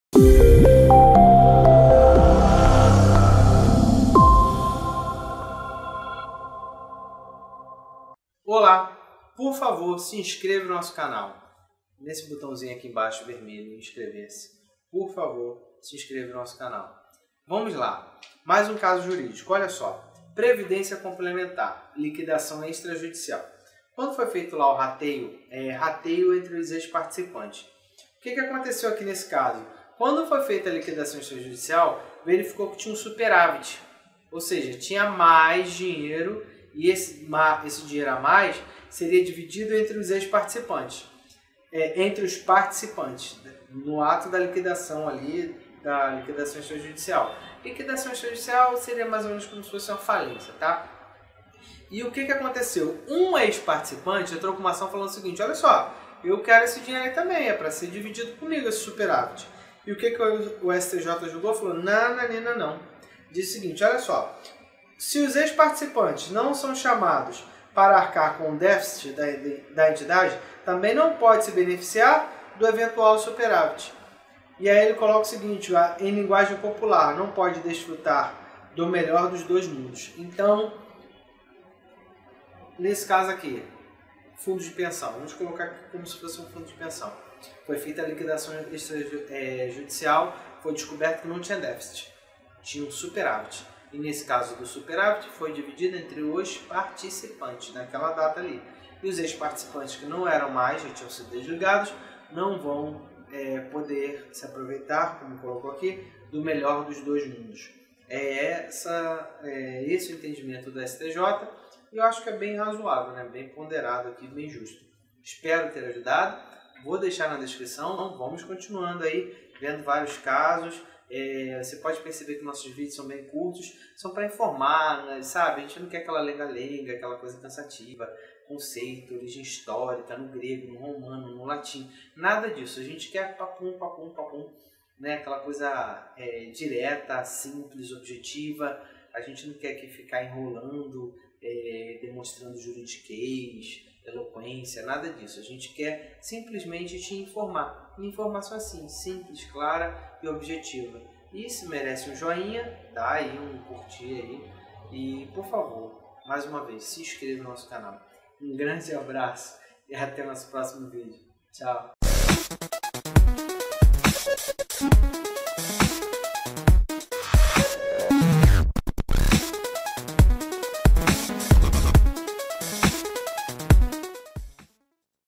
Ex-participante Recebe Superávit
O desligamento de participante de plano de previdência complementar faz cessar o direito ao rateio
de eventual superávit de ativos apurados em posterior liquidação extrajudicial da entidade.
REsp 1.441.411-AM, Rel. Min. Paulo de Tarso Sanseverino, por
unanimidade, julgado em 10/04/2018, DJe 19/04/2018

Previdência complementar. Liquidação extrajudicial. Rateio de ativos.
Inclusão de ex-participantes. Descabimento.

A questão central devolvida ao conhecimento desta Corte diz respeito à possibilidade de os
ex-participantes de plano de previdência complementar serem beneficiados pelo rateio de ativos da
entidade da qual eram filiados. Inicialmente, é importante pontuar que a condição de participante de
um plano de previdência complementar traz consigo ônus e bônus. Exemplo de bônus é o rateio de
superávit, que é a hipótese dos autos. Exemplo de ônus é ter de contribuir para a satisfação de
eventual déficit da entidade de previdência, conforme previsto no art. 21 da Lei Complementar n.
109/2001. Esse enunciado normativo prevê que o déficit da entidade de previdência deve ser
suportado conjuntamente pelos patrocinadores, participantes e assistidos, tendo apenas direito de
regresso contra os eventuais responsáveis pelo prejuízo do plano. Os ex-participantes, portanto, não
são chamados a arcar com o déficit da entidade de previdência. Deveras, o resgate das contribuições
faz cessar o vínculo do participante com a entidade de previdência. Ora, se os ex-participantes não
são chamados a arcar com o déficit da entidade, também não podem se beneficiar de eventual
superávit, pois isso equivaleria ao que se diz, em linguagem popular, desfrutar do "melhor dos dois
mundos".